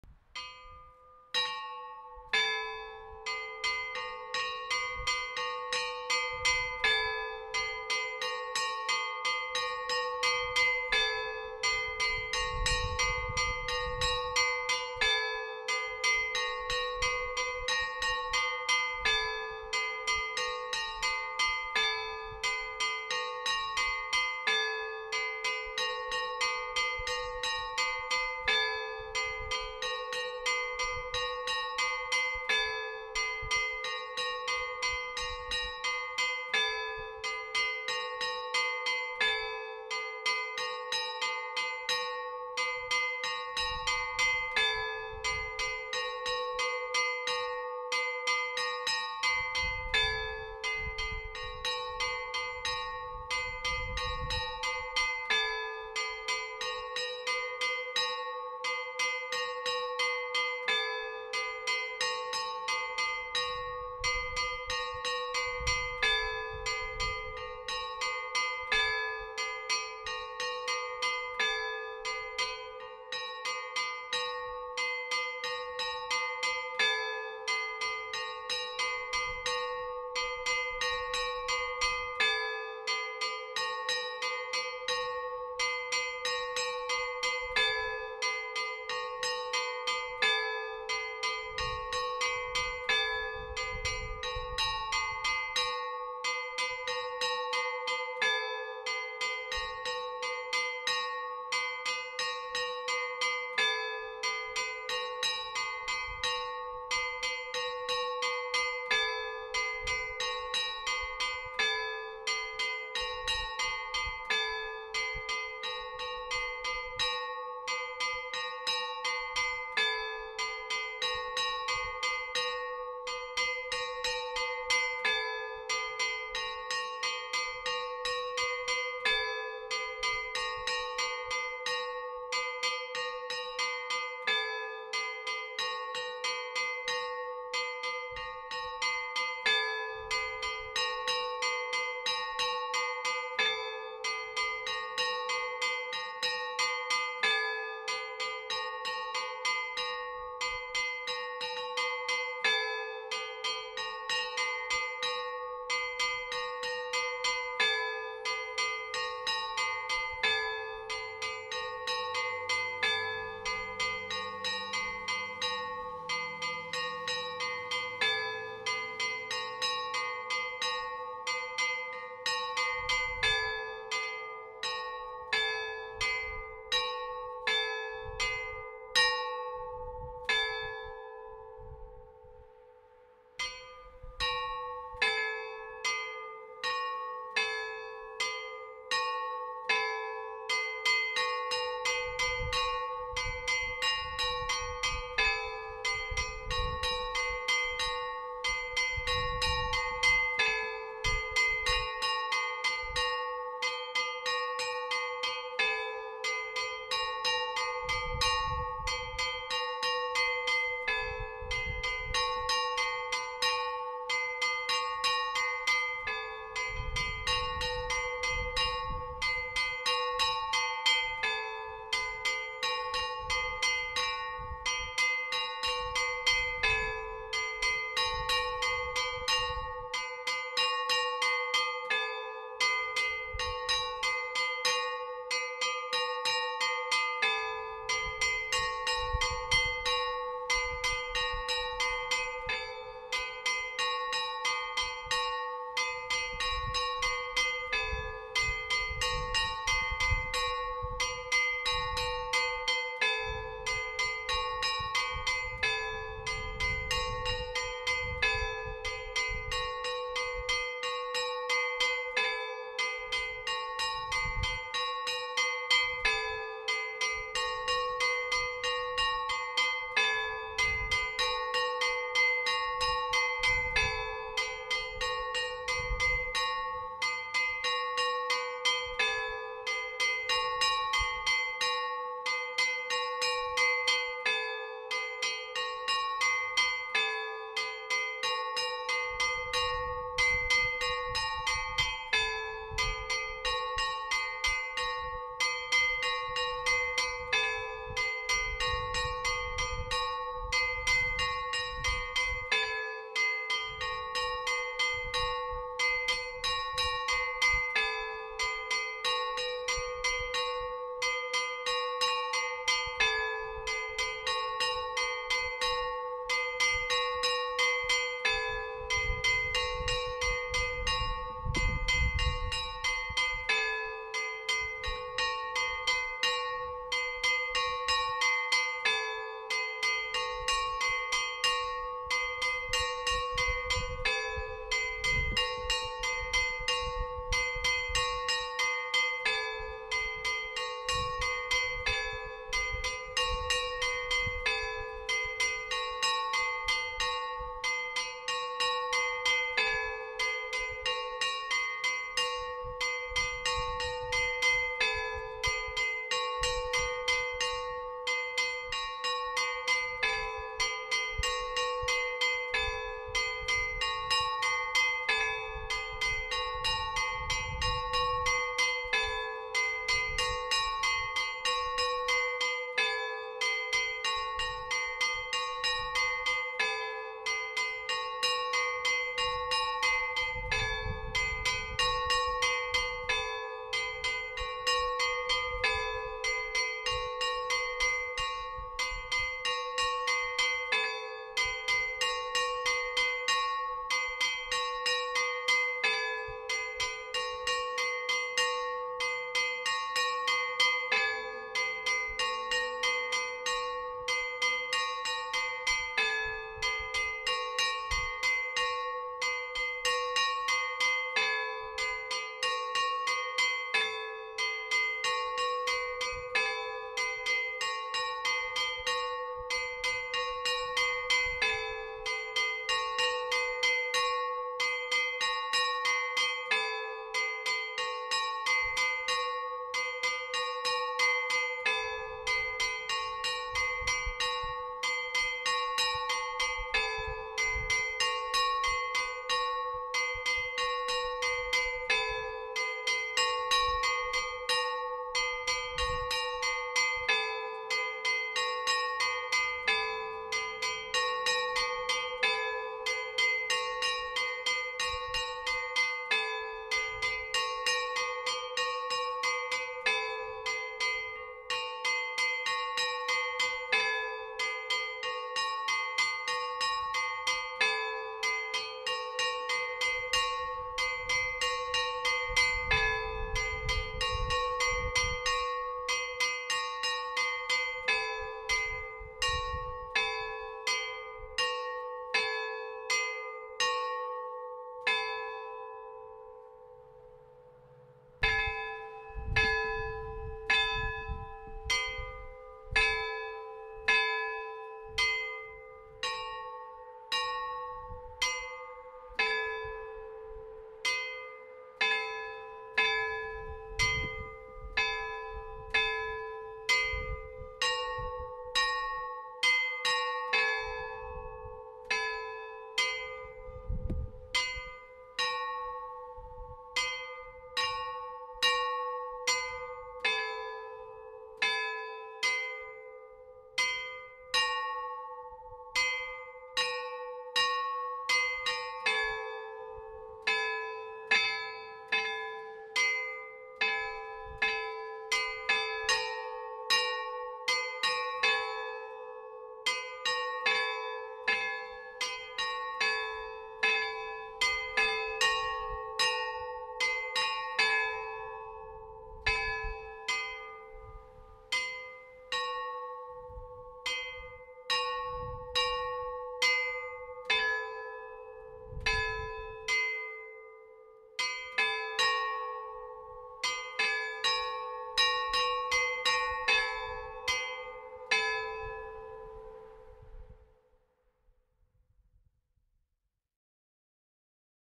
Scampanottata a Fiumicino (Pordenone)
Concerto di tre campane in Lab4 crescente fuse da Lucio Broili nel 1956. Scampanottata sul concerto.